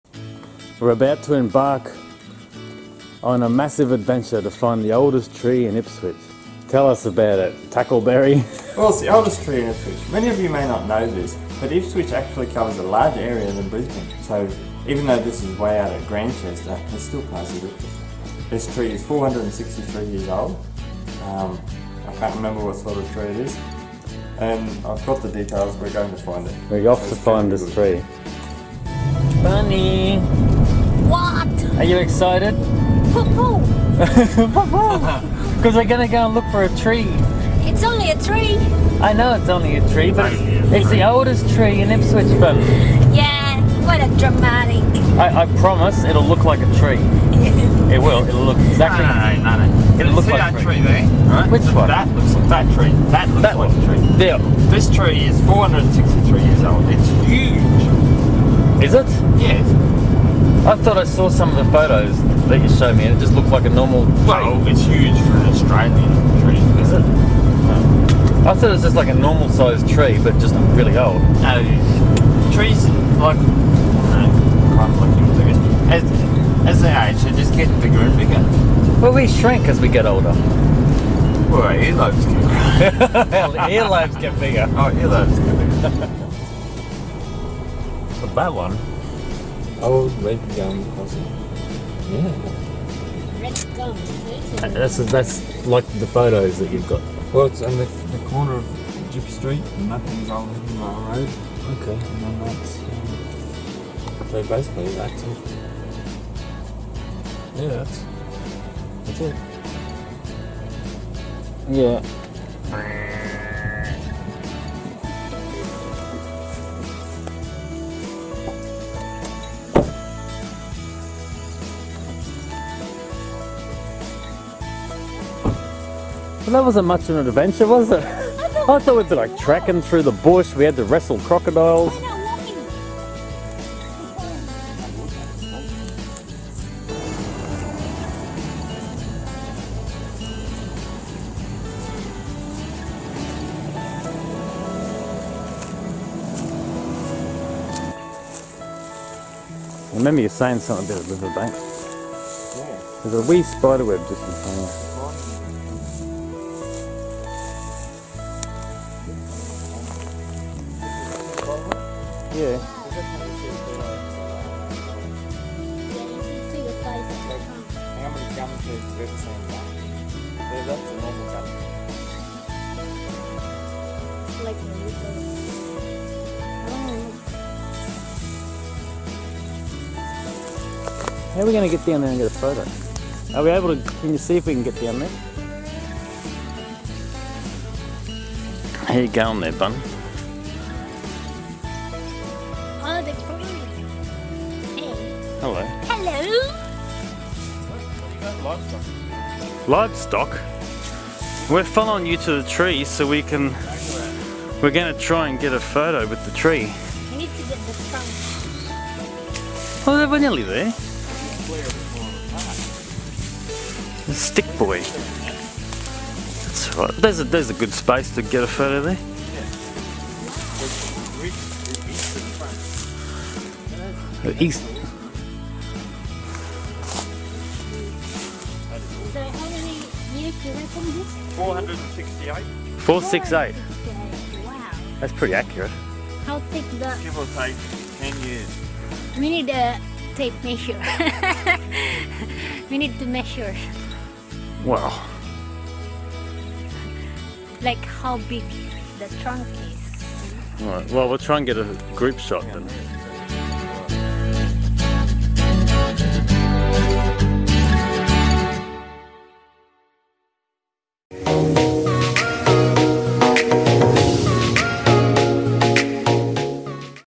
Old Tree Adventure
We went searching for the oldest tree in Ipswich. Had some fun and found it easily ha ha.

If you would like to make a donation to our charity
work, just go to the home page and click on "about."